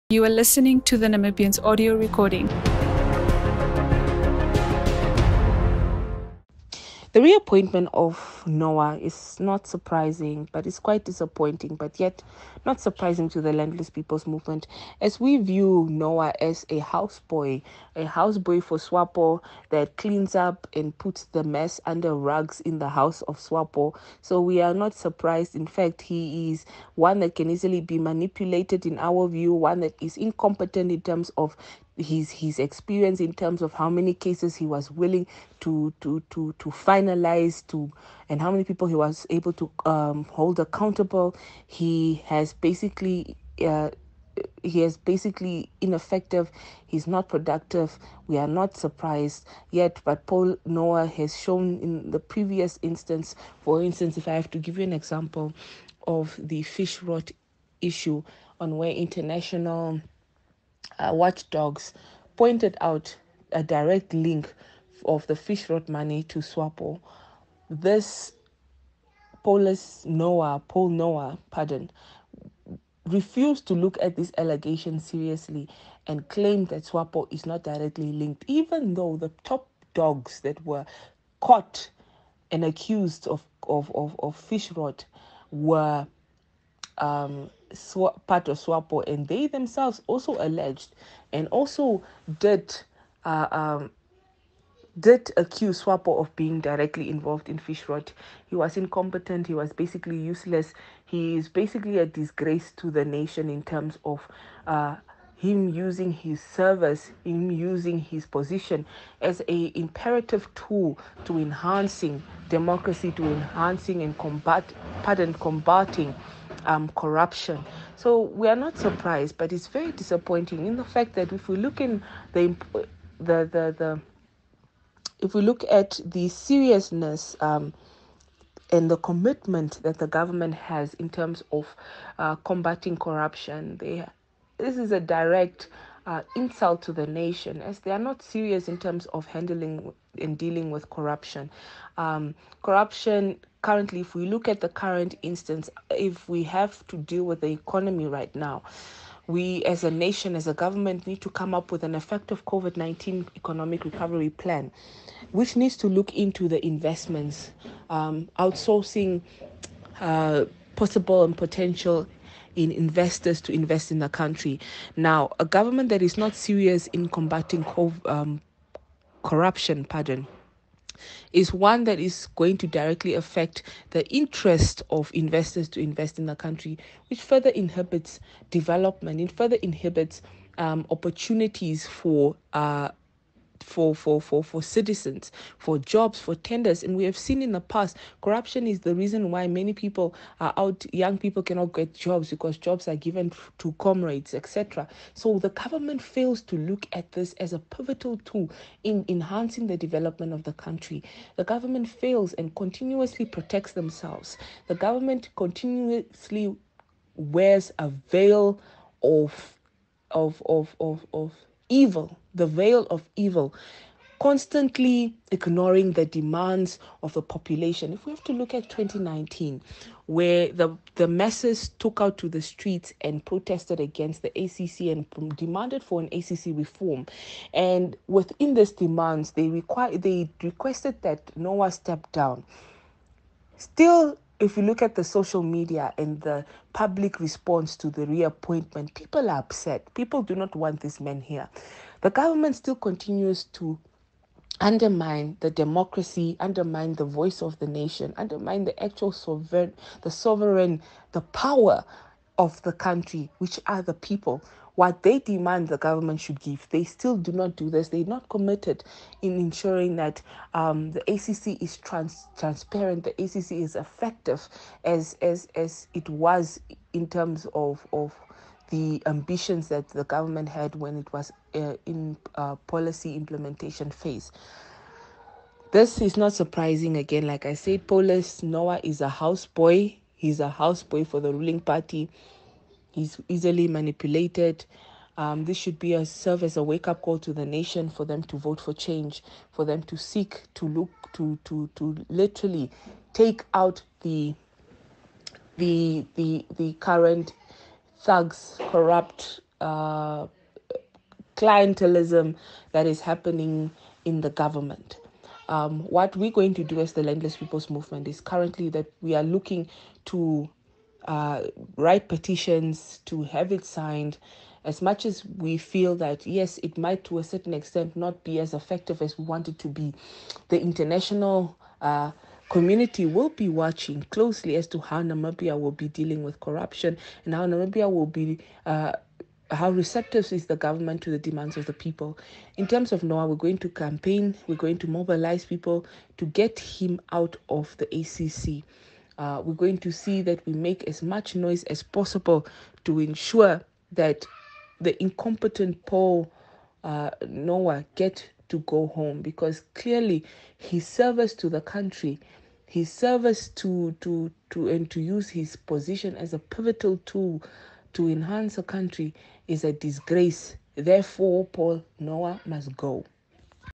Noa's reappointment an 'insult to the nation'
LPM parliamentarian Utaara Mootu said Geingob’s decision shows that the government continues to undermine the voices of the people. “Noa has been incompetence. He was basically useless. It is very disappointing and this is a direct insult to the nation. It shows that the government is not serious about fighting corruption,” she said.